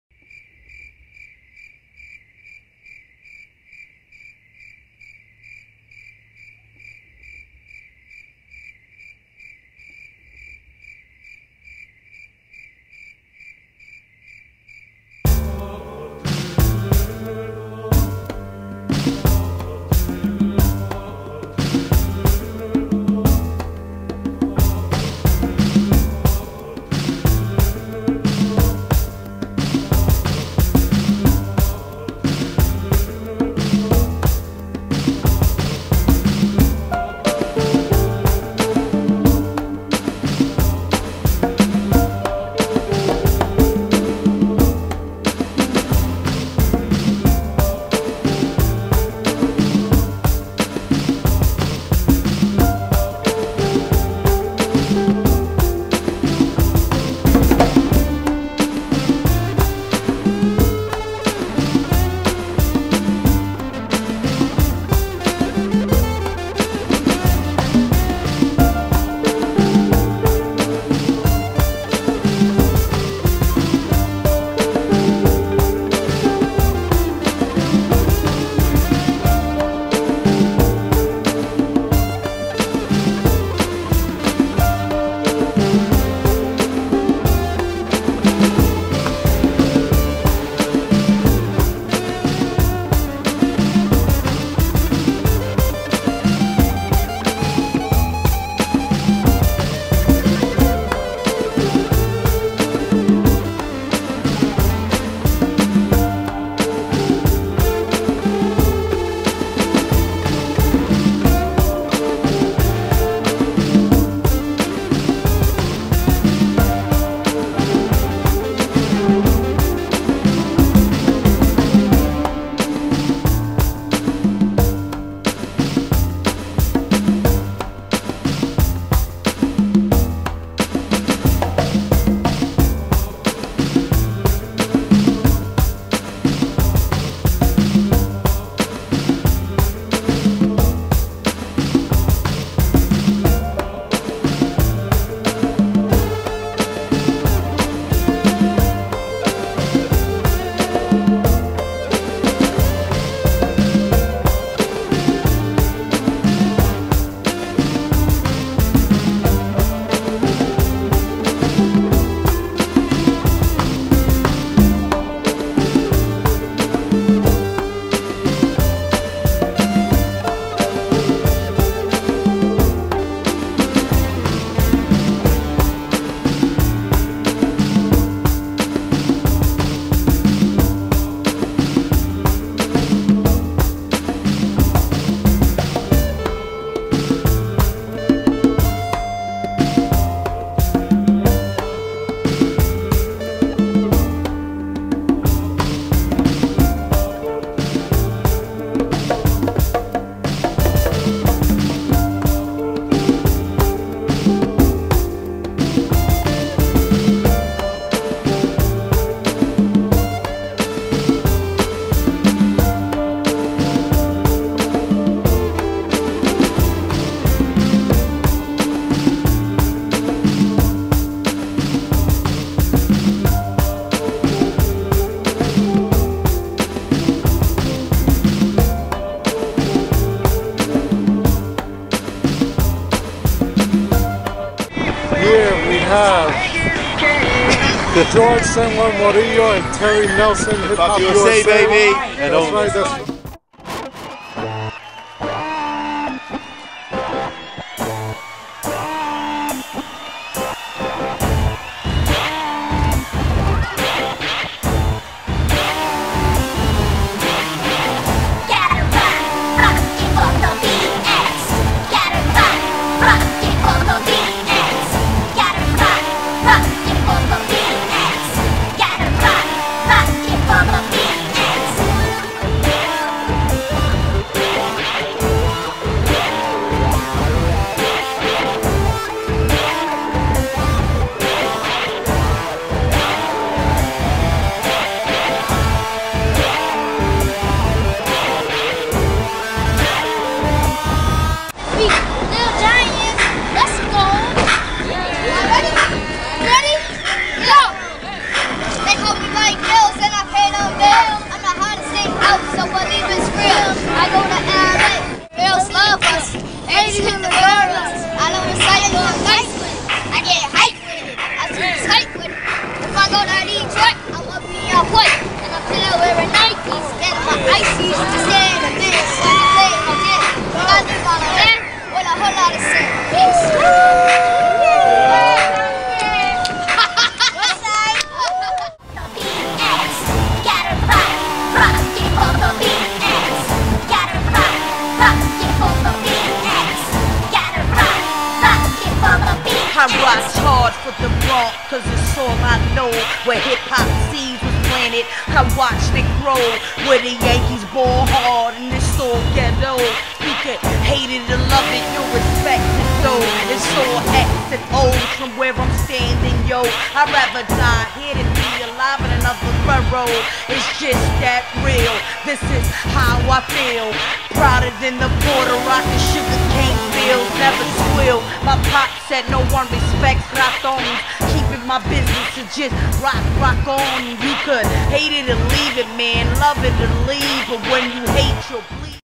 Artists Occupy BROW NYC 2011
Lafayette Street Holiday Block Party & Art Sale. Live Artist George "SEN One" Morillo, Gia Gutierrez, and SinXero.
Hip Hop USA, Terry Nelson...